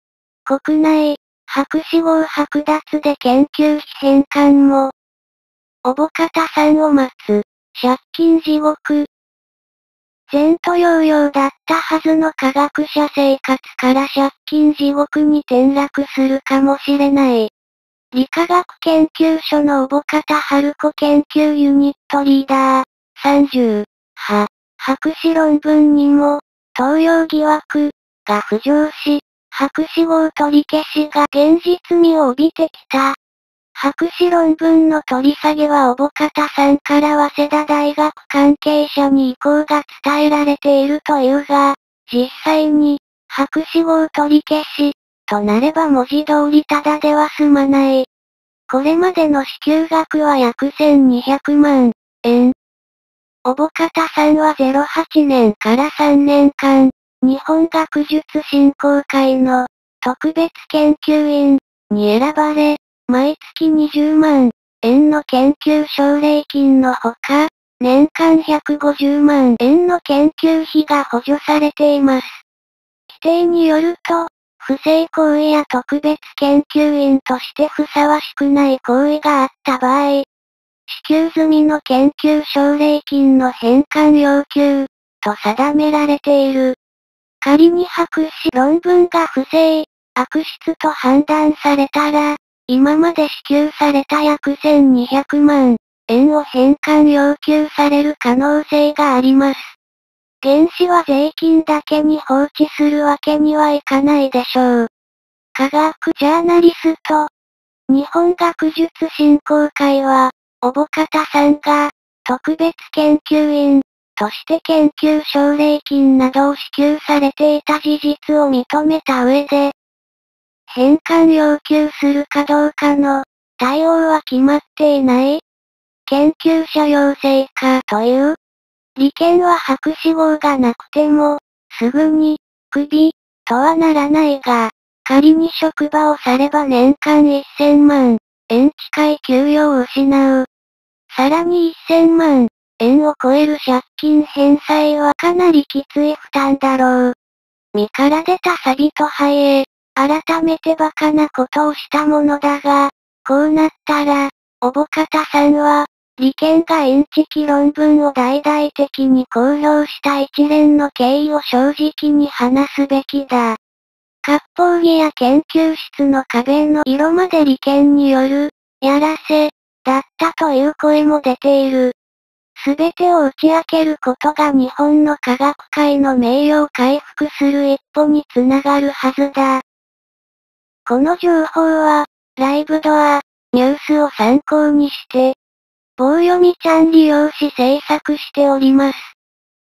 【国内】博士号剥奪で研究費返還も...小保方さんを待つ「借金地獄」
前途洋々だったはずの科学者生活から借金地獄に転落するかもしれない。理化学研究所の小保方晴子研究ユニットリーダー（３０）は、博士論文にも「盗用疑惑」が浮上し、博士号取り消しが現実味を帯びてきた。
博士論文の取り下げは小保方さんから早稲田大学関係者に意向が伝えられているというが、実際に「博士号取り消し」となれば文字通りタダでは済まない。
■これまでの支給額は約１２００万円
「小保方さんは０８年から３年間、日本学術振興会の『特別研究員』に選ばれ、毎月２０万円の研究奨励金のほか、年間１５０万円の研究費が補助されています。規定によると、不正行為や特別研究員としてふさわしくない行為があった場合、『支給済みの研究奨励金の返還要求』と定められている。仮に博士論文が不正、悪質と判断されたら、今まで支給された約１２００万円を返還要求される可能性があります。原資は税金だけに放置するわけにはいかないでしょう」（科学ジャーナリスト）
日本学術振興会は、小保方さんが「特別研究員」として研究奨励金などを支給されていた事実を認めた上で、「（返還要求するかどうかの）対応は決まっていない」（研究者養成課）という。
理研は博士号がなくても、すぐに「クビ」とはならないが、仮に職場を去れば年間１０００万円近い給与を失う。さらに１０００万円を超える借金返済はかなりキツイ負担だろう。
身から出たサビとはいえ、あらためて馬鹿なことをしたものだが、こうなったら、小保方さんは、理研がインチキ論文を大々的に公表した一連の経緯を正直に話すべきだ。割烹着や研究室の壁の色まで理研による「ヤラセ」だったという声も出ている。すべてを打ち明けることが日本の科学界の名誉を回復する一歩につながるはずだ。

この情報は、livedoor ニュースを参考にして
棒読みちゃん利用し制作しております。